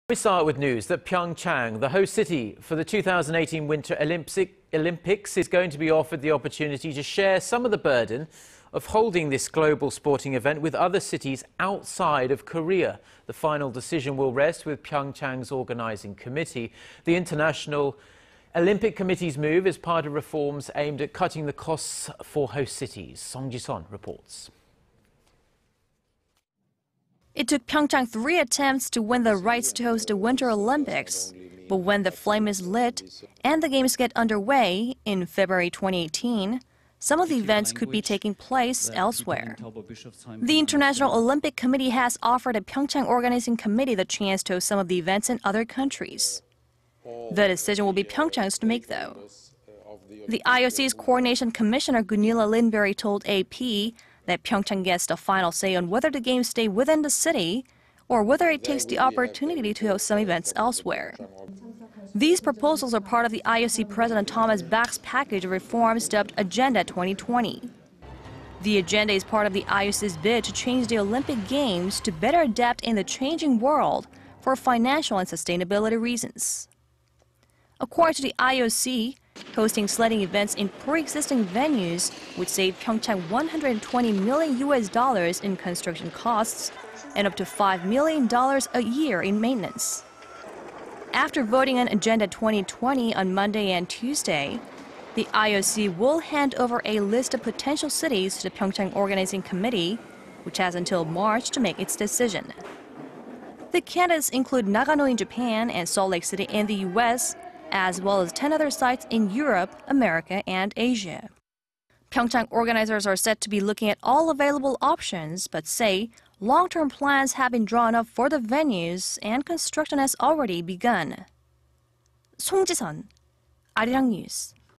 IOC tells PyeongChang it can hold some 2018 Olympic events in other cities if it
And we start with news that PyeongChang... the host city for the 2018 Winter Olympics is going to be offered the opportunity to share some of the burden of holding the global sporting event with other cities outside of Korea.
The final decision will rest with PyeongChang′s organizing committee.
The International Olympic Committee′s move is part of reforms aimed at cutting the costs for host cities.
Song Ji-sun reports.
It took PyeongChang three attempts to win the rights to host the Winter Olympics... but when the flame is lit... and the Games get underway... in February 2018... some of the events could be taking place elsewhere.
The International Olympic Committee has offered the PyeongChang Organizing Committee the chance to host some of the events in other countries.
The decision will be PyeongChang′s to make though.
The IOC′s coordination commissioner Gunilla Lindberg told AP that PyeongChang gets the final say on whether the Games stay within the city or whether it takes the opportunity to host some events elsewhere.
These proposals are part of IOC President Thomas Bach′s package of reforms dubbed ′Agenda 2020.′
The agenda is part of the IOC′s bid to change the Olympic Games to better adapt in the changing world, for financial and sustainability reasons.
According to the IOC, hosting sledding events in pre-existing venues would save PyeongChang 120 million U.S. dollars in construction costs... and up to five million dollars a year in maintenance.
After voting on ′Agenda 2020′ on Monday and Tuesday,... the IOC will hand over a list of potential cities to the PyeongChang Organizing Committee... which has until March to make its decision.
The candidates include Nagano in Japan and Salt Lake City in the U.S., as well as ten other sites in Europe, America and Asia.
PyeongChang organizers are said to be looking at all available options, but say long-term plans have been drawn up for the venues and construction has already begun.